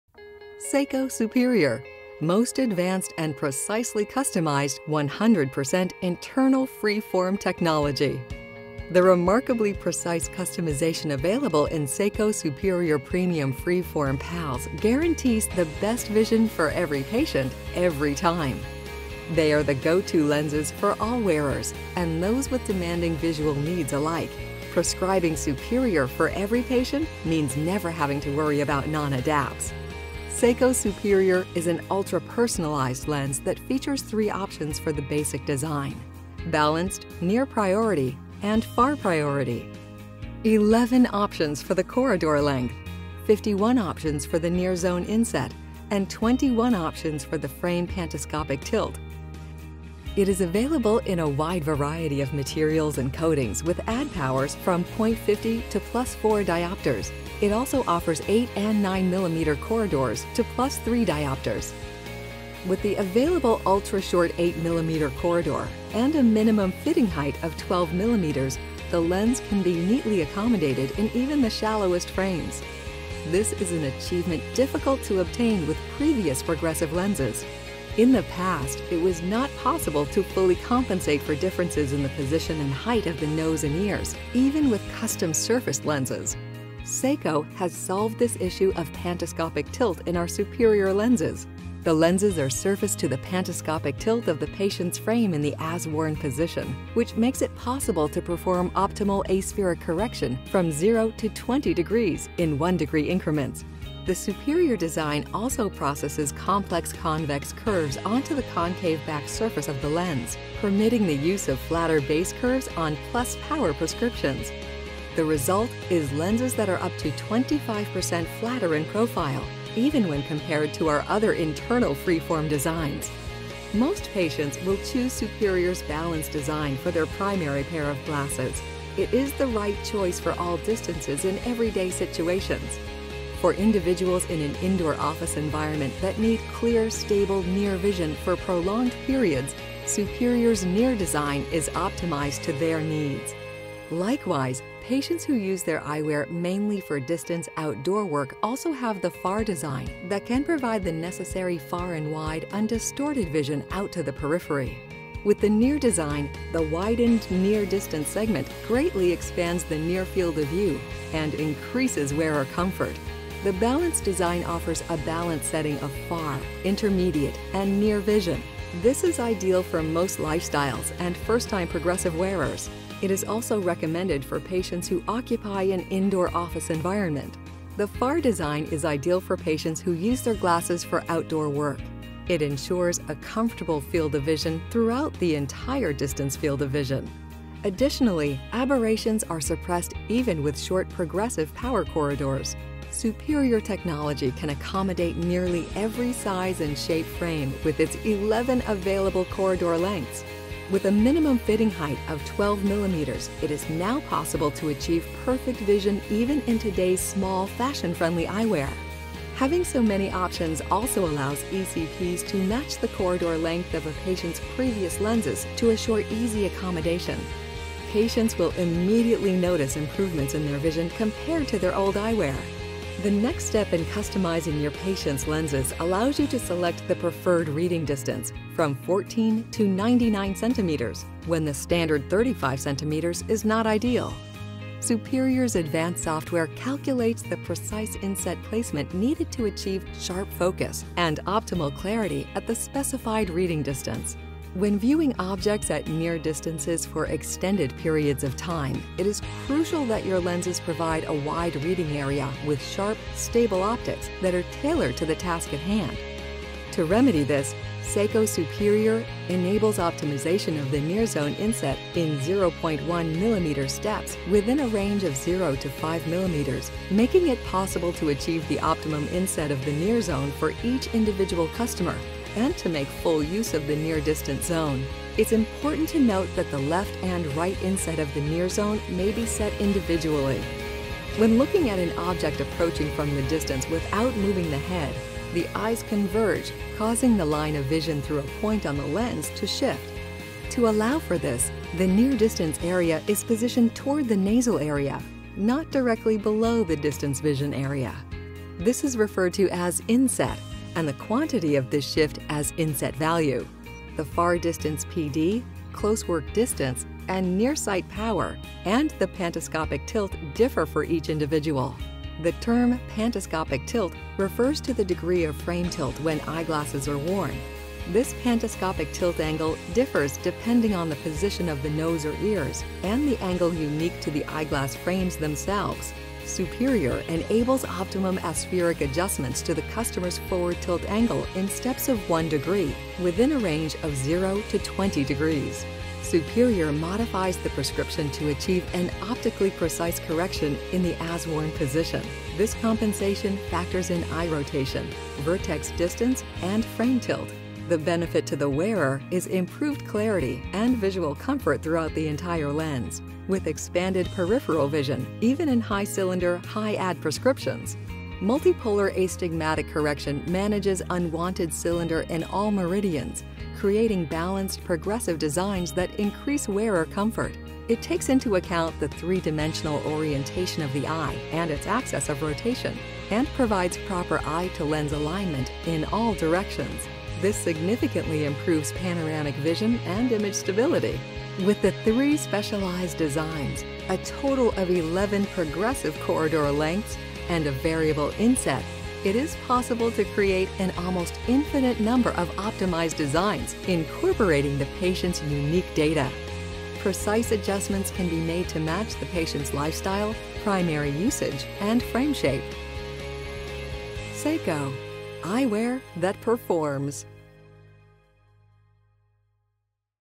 Plastic Plus and Seiko Superior Lens Technology
SEIKO Superior is an “ultra-personalized lens” that features 3 options for the basic design (Bal­anced, Near-priority and Far-priority), 11 options for the corridor length, 51 options for the near zone inset and 21 options for the frame pantoscopic tilt. It is available in a wide variety of materials and coatings with add powers from +0.50 to +4.00 (8 & 9mm corridors to +3.00).